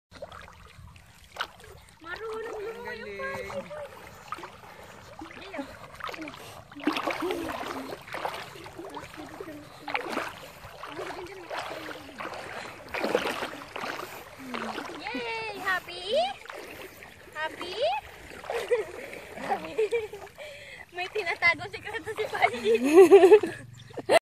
Shih Tzu Dogs First Time swimming in the Sea / Fudgee and Fiona
We went to our province which is Bicol last June 2017 to have a short vacation. My shih tzus namely Fudgee and Fiona enjoyed swimming in Binahian Beach located at the town of Sipocot, Camarines Sur. It was their first time and they loved it!